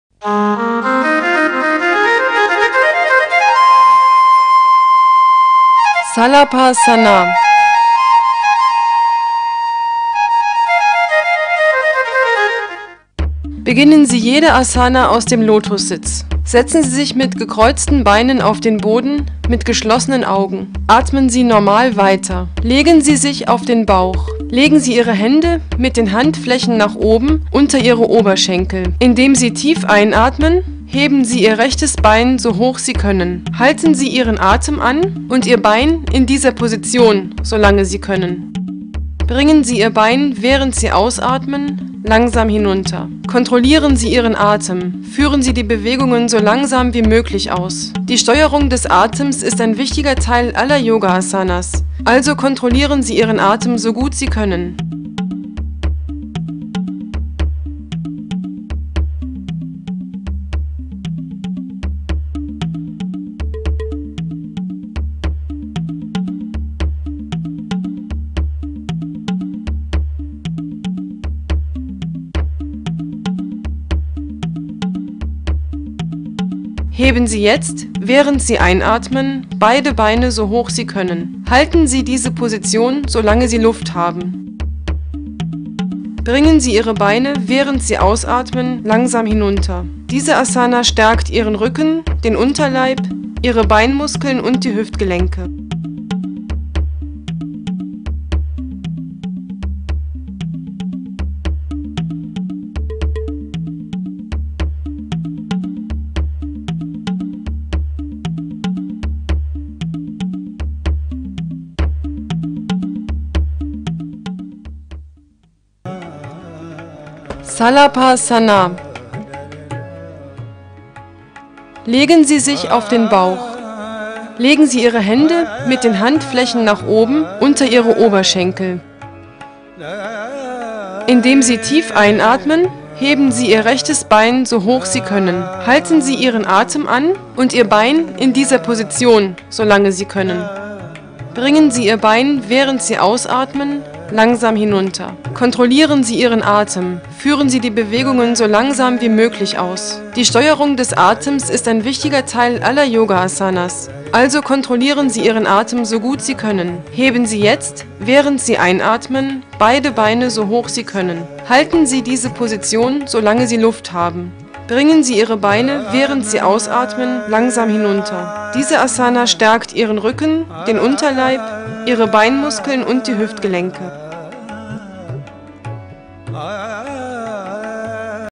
Salabhasana Yoga For The Young At Heart In Deutsch
Shalabhasana;
    This asana resembles the pose of Locust or Grasshopper. Hence this is called Shalabhasana. First rest in Makarasana. Then keep the hands below thighs and raise up both the legs together. The legs should be as straight as possible. Inhale and remain In this posture for a few seconds and then relax. Repeat this three to six times initially. After gaining sufficient experience you can remain in Shalabhasana for a longer duration, at a stretch. However do not strain beyond your capacity. For easy practice start with each leg separately for six times. Then you can try raising both the legs.

Benefits:Shalabhasana is useful for stomach., Lumber, waist portions and to the legs. Practice of this asana removes constipation. The waist remains trim if you practice Shalabhasana regularly.